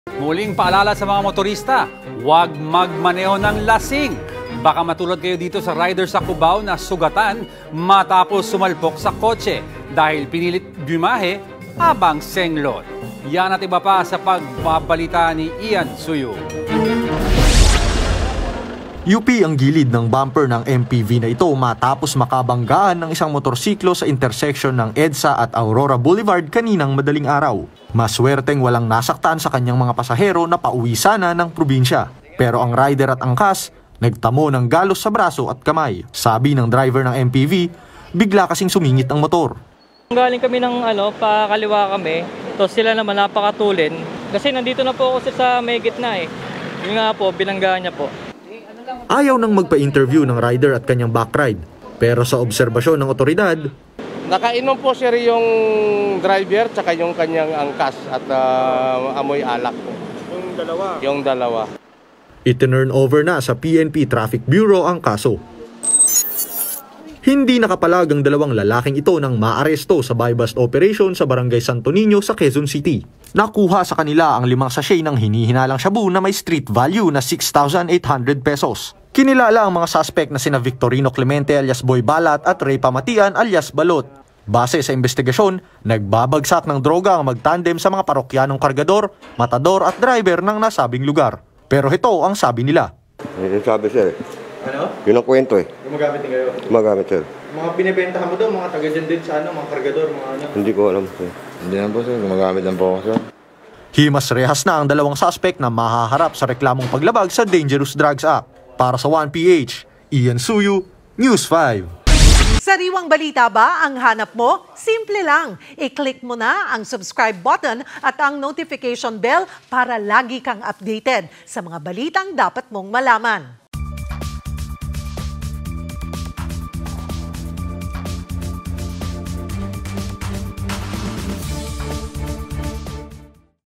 Rider at angkas, sugatan matapos bumangga sa kotse
News Balita LiveNewsTodayPhilippinesOnePH Sugatan ang isang rider at angkas nito matapos bumangga sa isang kotse sa Quezon City. Ang mga nasa motor, nakainom daw!| via Ian Suyu